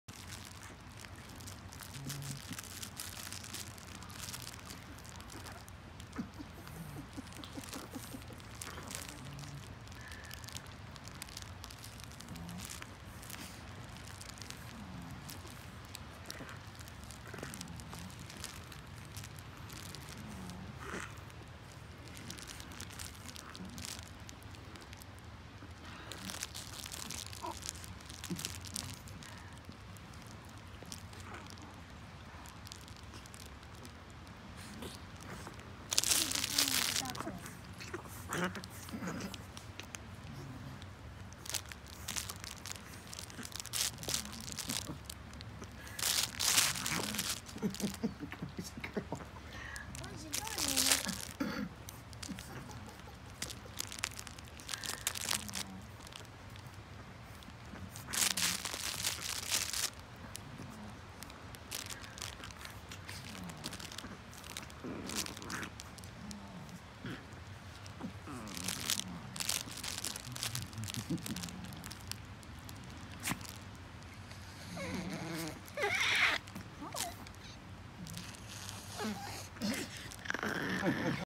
Taco Tuesday, chihuahua style
Chihuahua puppies playing with a crunchy taco toy!